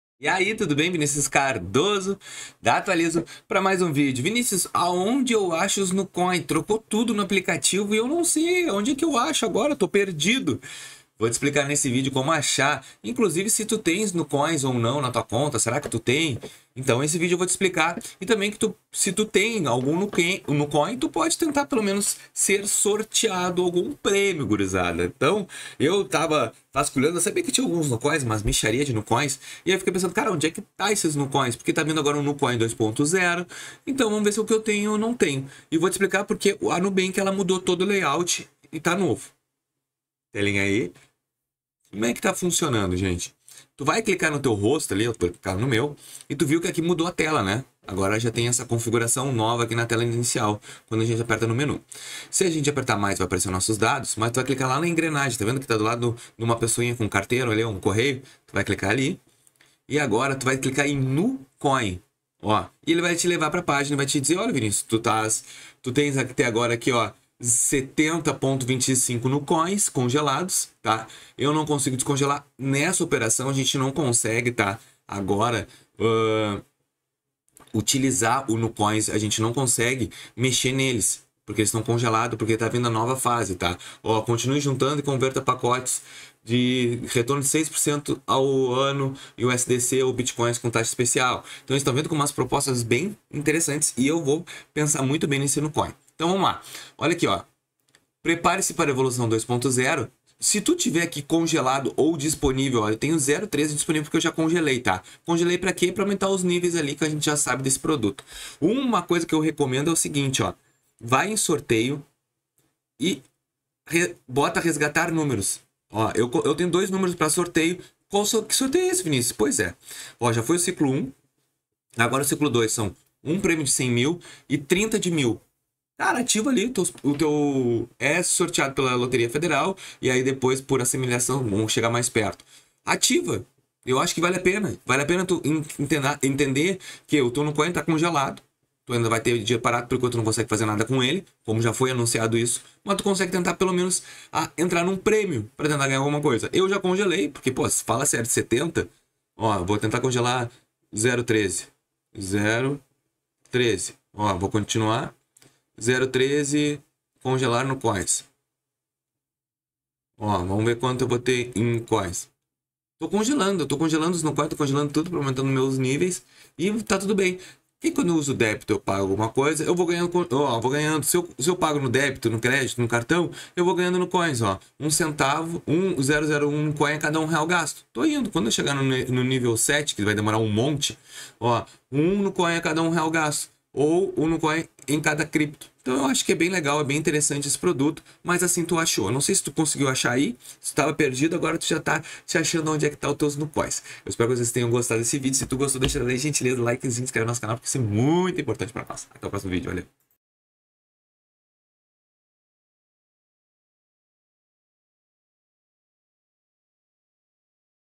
NUCOINS PERDIDOS - Como ACHAR MEUS NUCOINS?
NUCOINS PERDIDOS - Como ACHAR MEUS NUCOINS? @Nubank
Perdeu seus Nucoins no app do Nubank e não sabe onde encontrá-los? Neste vídeo, explico o passo a passo para localizar seus Nucoins, verificar o saldo e acompanhar suas recompensas acumuladas. Descubra as possíveis causas do sumiço dos Nucoins e aprenda como acessar essas informações de forma rápida!

Direto ao Ponto:
00:00 - Início
00:30 - NUCOINS PERDIDOS - Como ACHAR MEUS NUCOINS?
03:11 - Encerramento